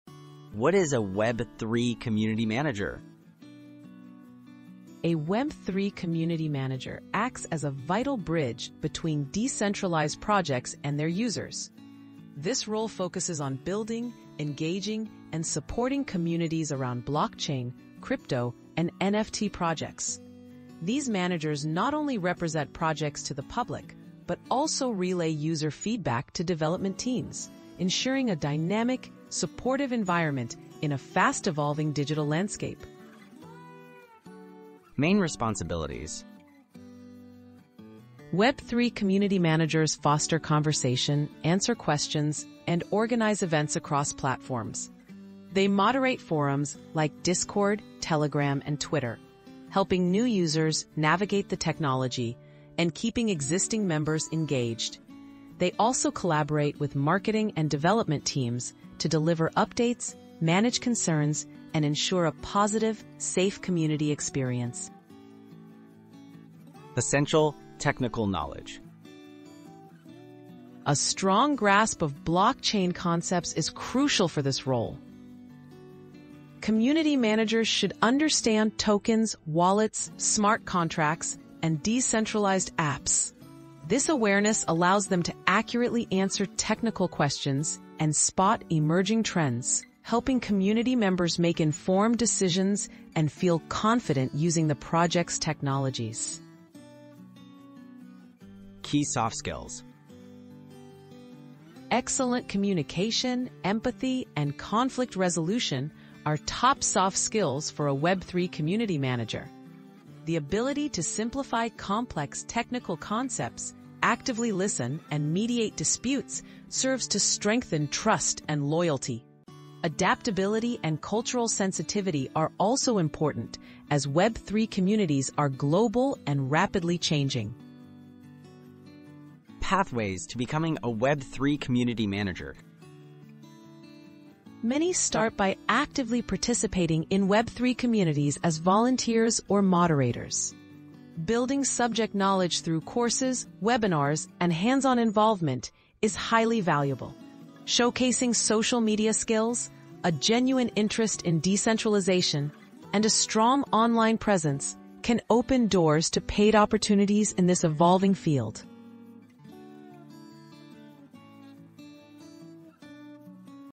Web3 Community Manager: Skills, Role, and Career Path
Web3 Manager Guide 👉 Discover what it takes to become a successful Web3 community manager! Learn the core responsibilities, must-have technical knowledge, and essential soft skills for this high-demand role in blockchain, crypto, and NFT projects. Perfect for anyone aiming to start or advance their Web3 career.

00:00 🔹 What Is a Web3 Community Manager?
00:34 🔹 Main Responsibilities
01:06 🔹 Essential Technical Knowledge
01:38 🔹 Key Soft Skills
02:09 🔹 Pathways to Becoming a Web3 Community Manager

📌 Emerging Tech Career Profiles 🔸 Blockchain & Web3 Job Opportunities 🔸 Web3 Community Manager: Role Overview and Required Skills